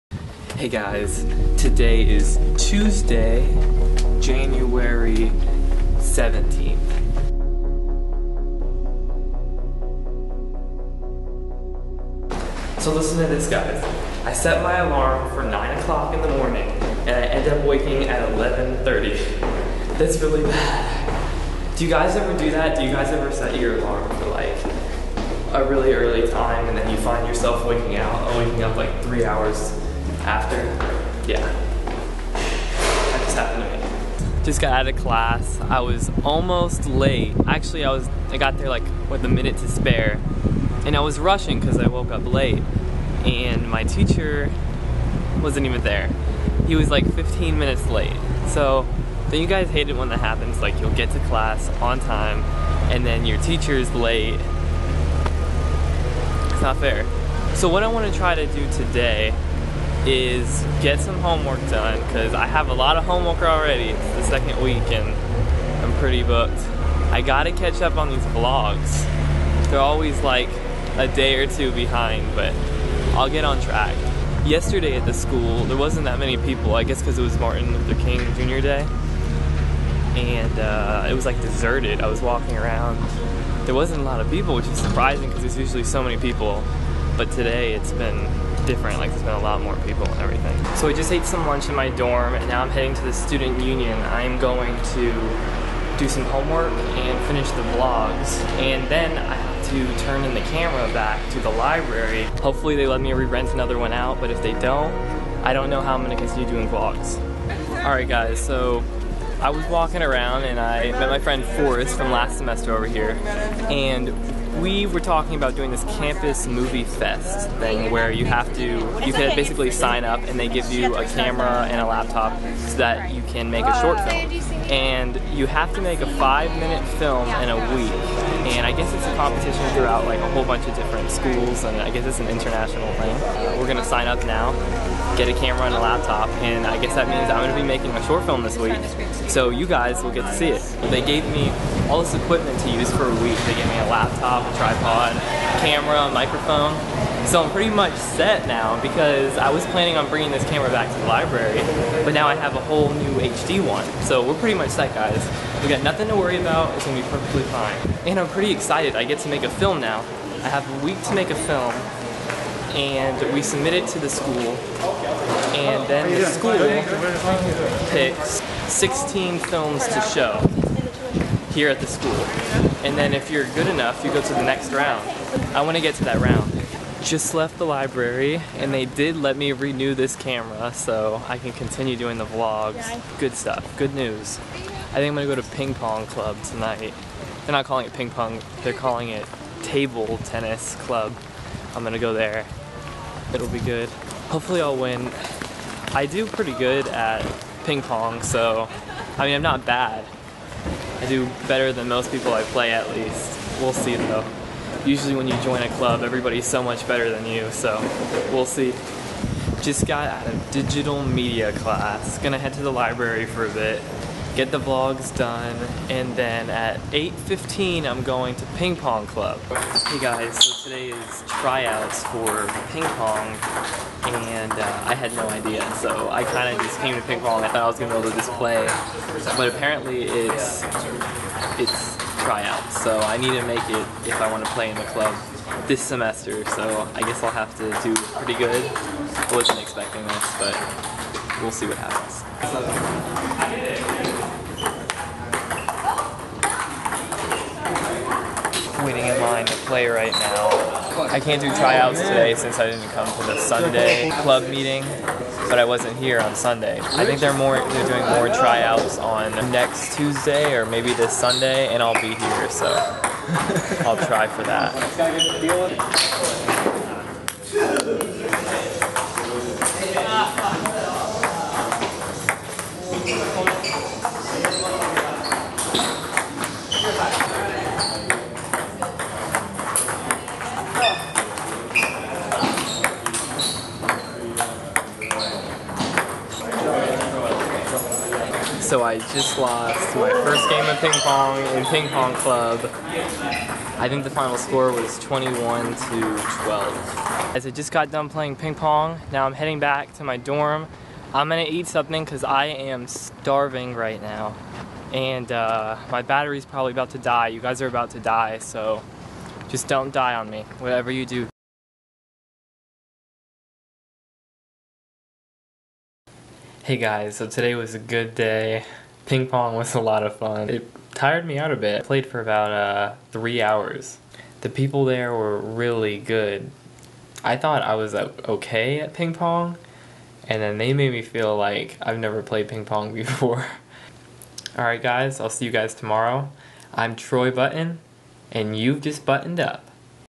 Button Up 1.17.12 "Ping Pong Club"
Today I catch up on vlogs and attend the UCF Ping Pong Club!